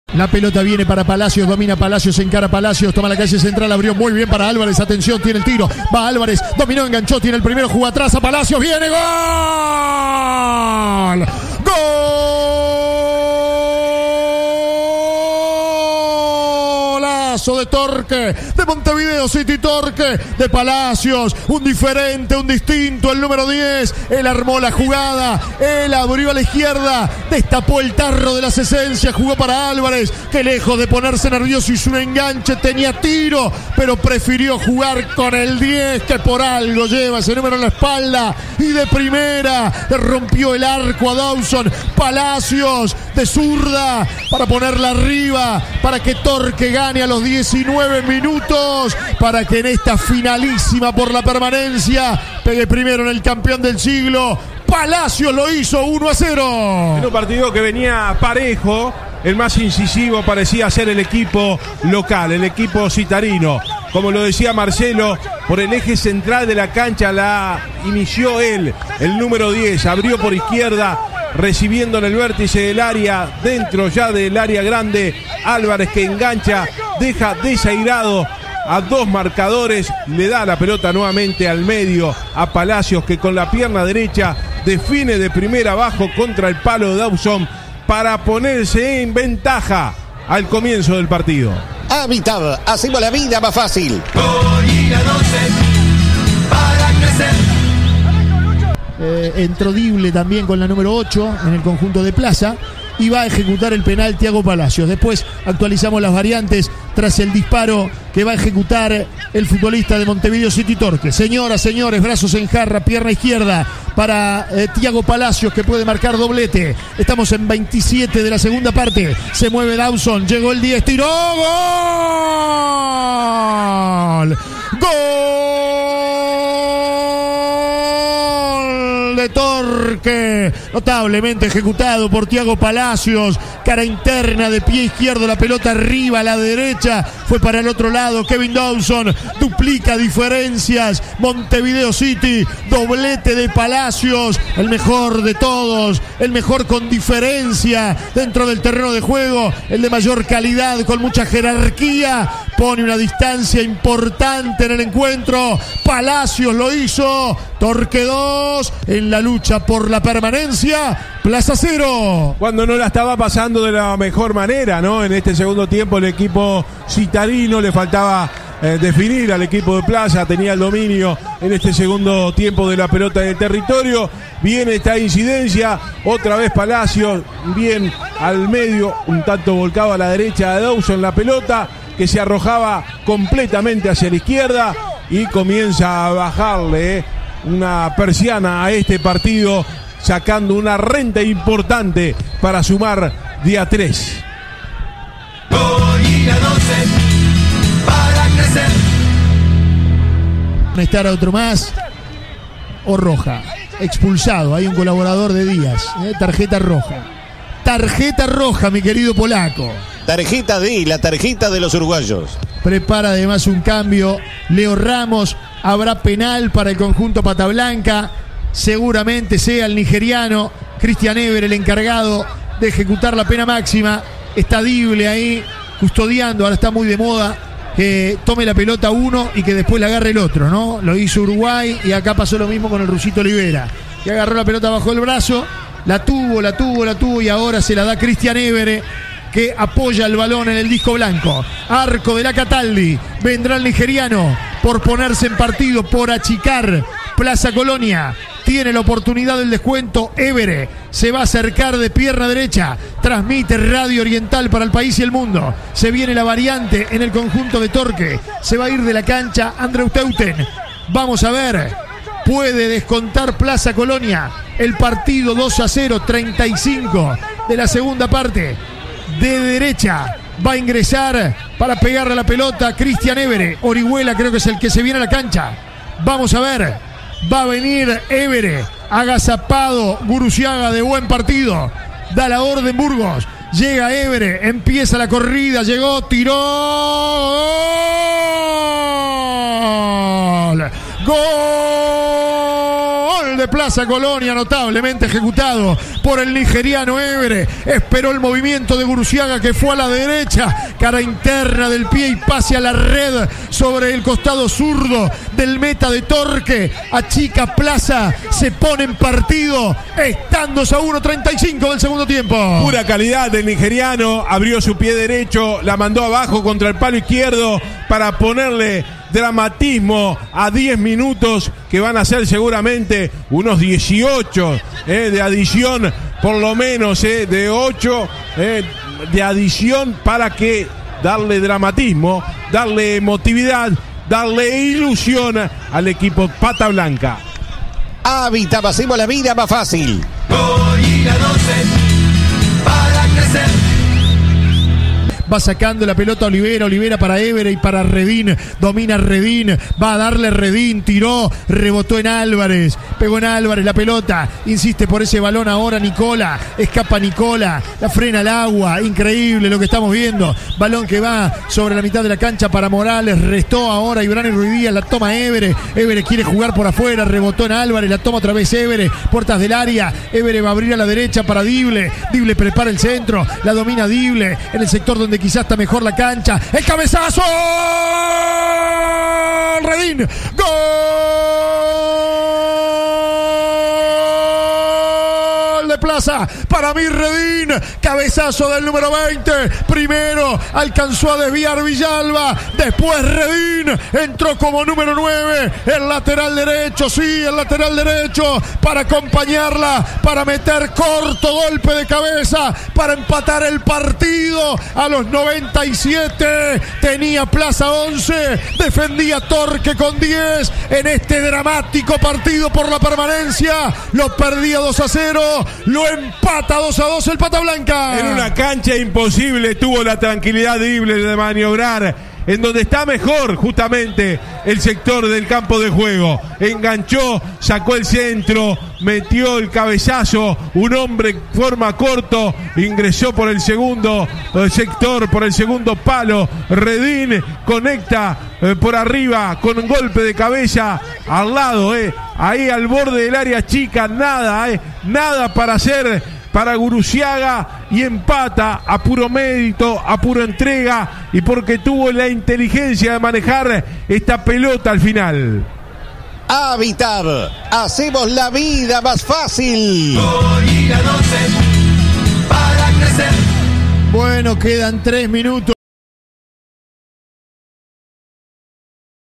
Torque 2 Plaza Colonia 2 - Clausura 2023 (por Marcelo Sanzó)
Los goles del partido, por Marcelo Sanzó y Fernando Pérez para CX 12 Radio Oriental 770 AM.